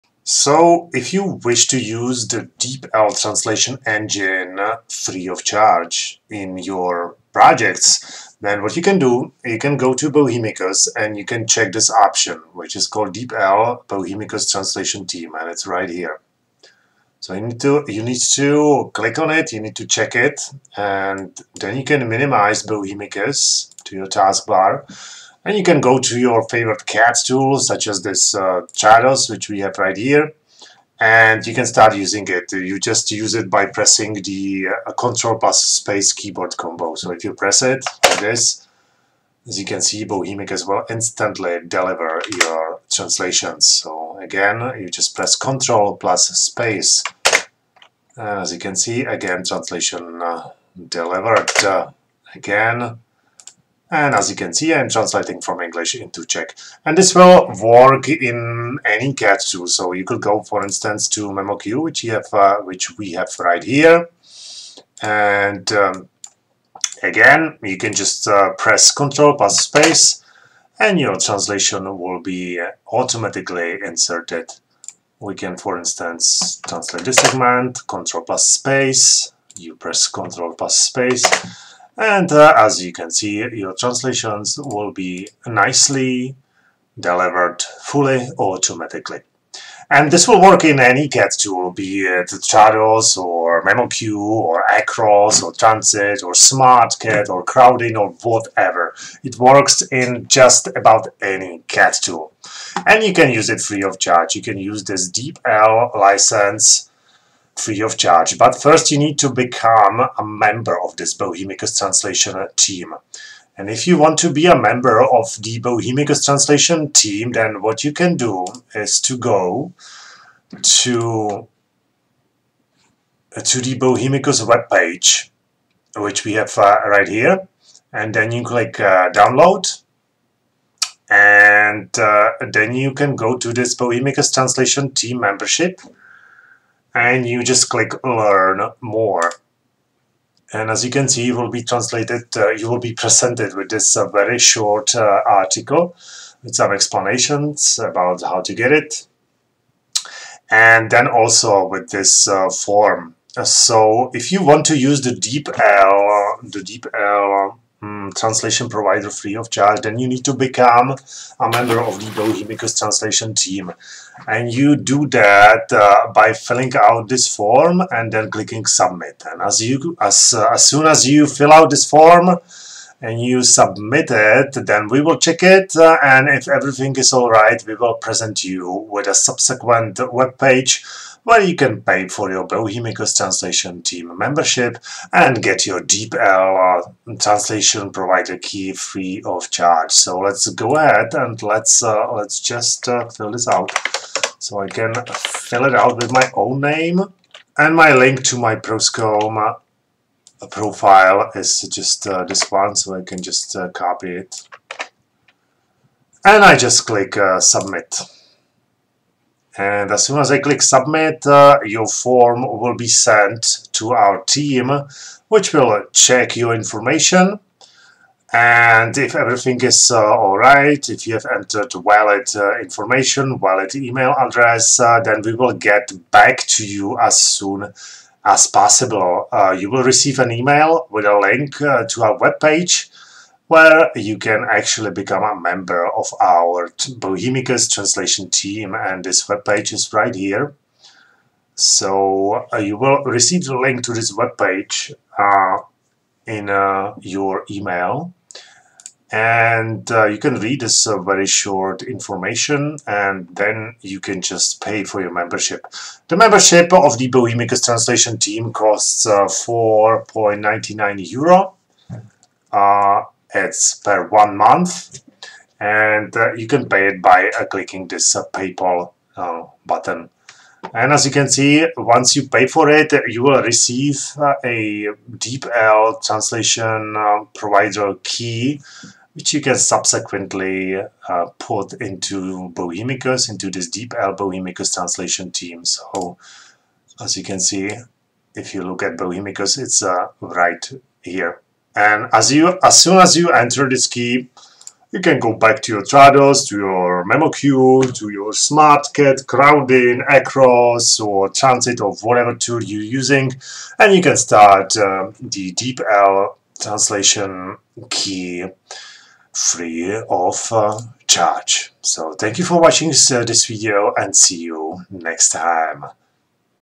Free DeepL in any CAT tool as a Bohemicus Team Member
Learn how to use DeepL in any CAT tool as a Bohemicus Team Member free of charge. Simply fill out the form, let us check it, and use a DeepL licence for our as well as your own projects free of charge!